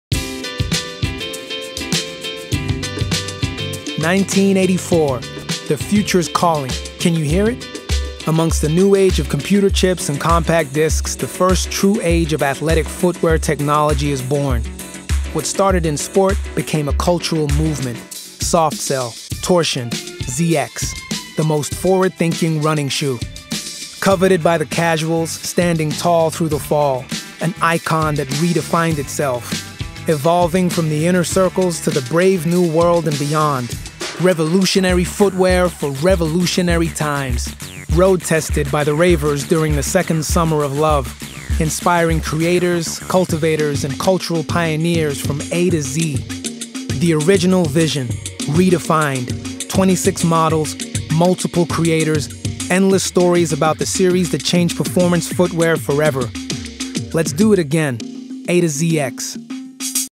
adidas A-ZX Educational Film
The ZX line changed the world of athletic footwear forever when it was introduced back in 1984. The original A-ZX series of 2008 features some of the most sought after shoes in existence and is known for being one of the best series of shoes ever. Beginning this year and ending in 2021, it returns!

A-ZX is a series of 26 tributes to the classic adidas running franchise, all of which will be available at HANON...